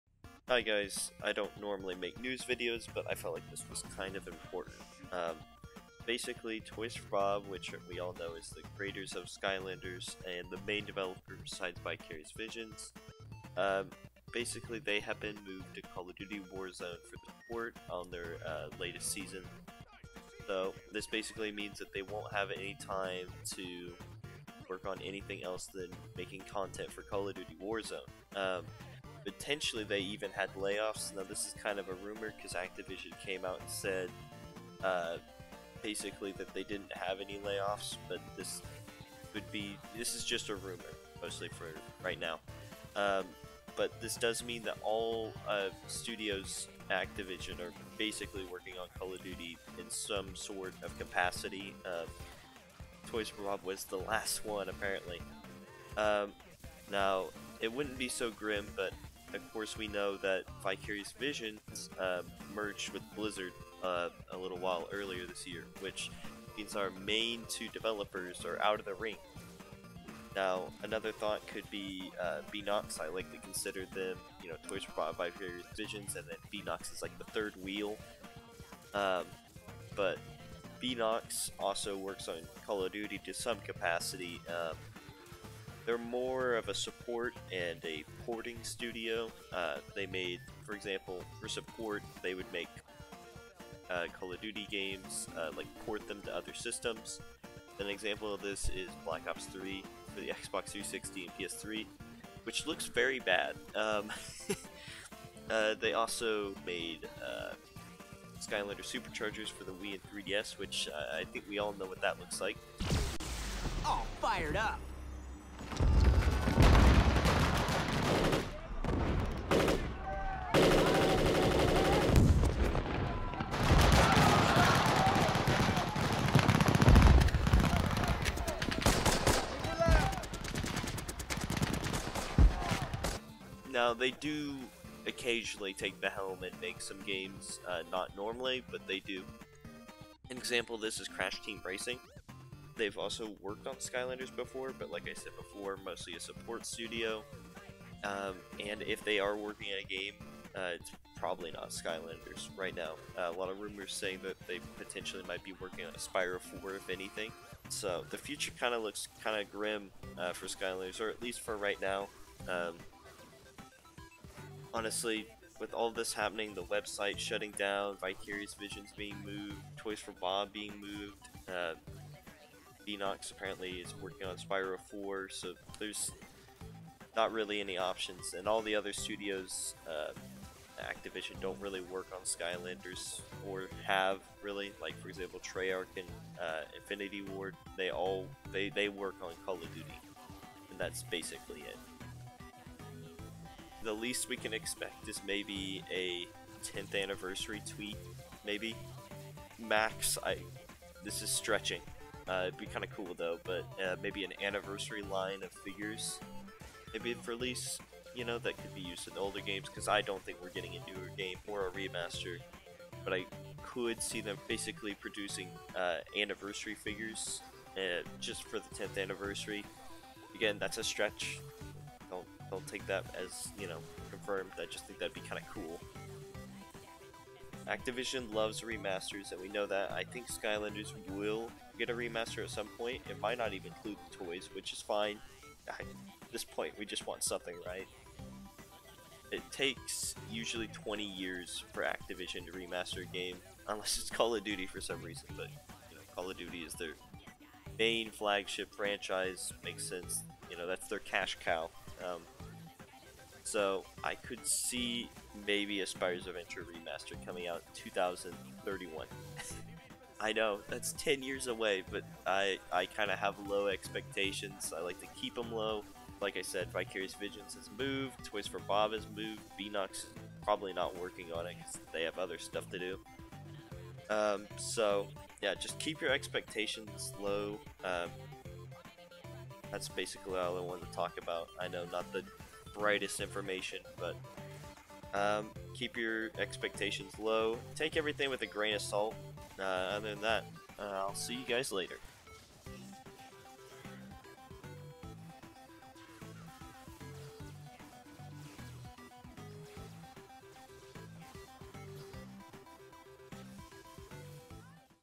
The Final Nail in the Skylanders Coffin
Toys for Bob has been moved to work on Call of Duty: Warzone and help with the latest season. This effectively kills all chances of a Skylanders 2021.